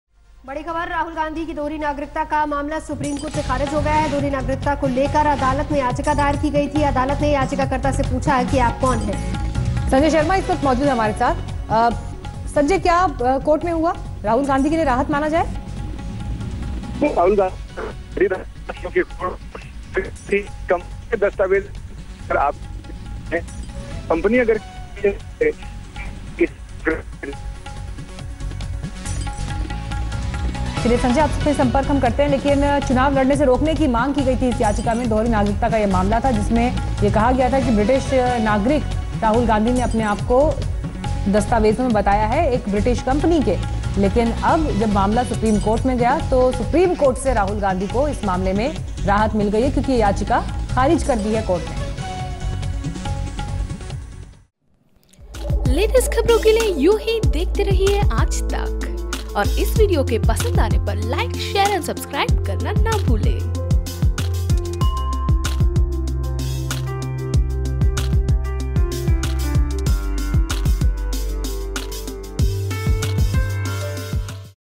नागरिता मामले में Rahul Gandhi के खिलाफ याचिका ख़ारिज !
नागरिकता मामले में राहुल को मिली सुप्रीम कोर्ट से राहत  दस्तावेज कम होने की वजह से नहीं होगा कुछ भी साथ ही मांगी गयी थी चुनाव न लड़ने देने की भी मांग वो भी हुई रद्द